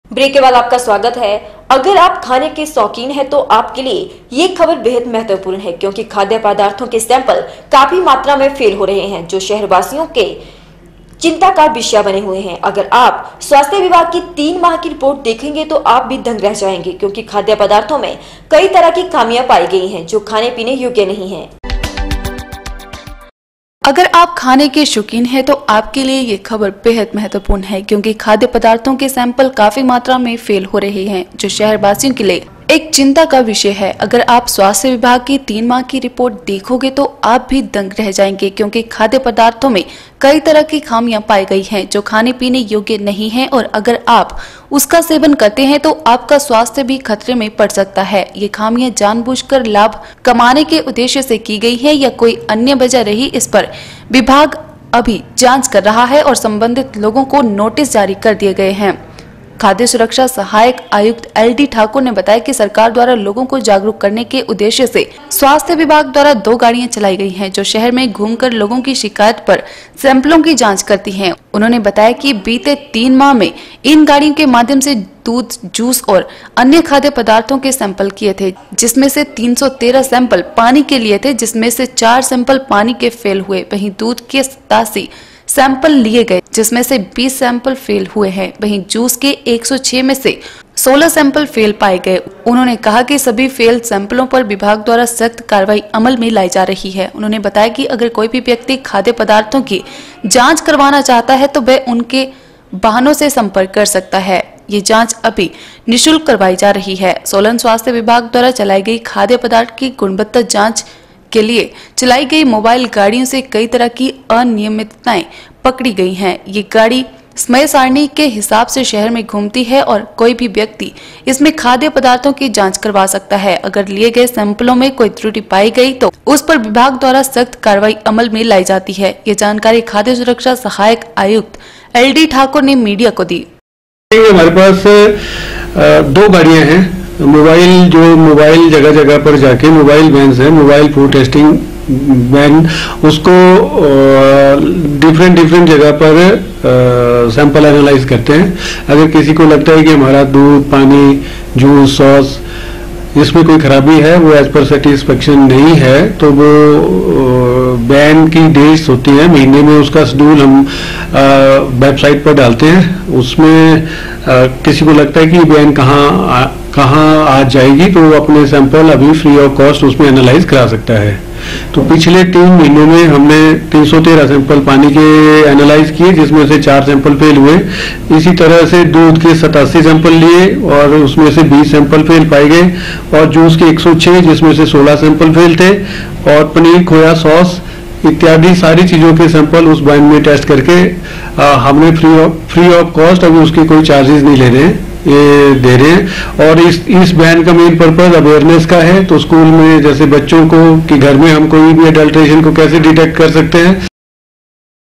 25 jan n 13 खाद्य पदार्थों मे कई तरह की खामियां पाई
अगर आप खाने के शौक़ीन है तो आप के लिए यह खबर बेहद महत्व पूर्ण है   क्योंकि खाद्य पदार्थों के सैम्पल काफी मात्रा में फेल हो रहे है  जो शहर वासियों के इए एक चिंता का विषय है | अगर आप स्वास्थ्य विभाग की तीन माह की रिपोर्ट देखोगे तो आप भी दंग रह जाएंगे | क्योंकि खाद्य पदार्थों मे कई तरह की खामियां पाई गई है जो खाने पीने योग्य नहीं है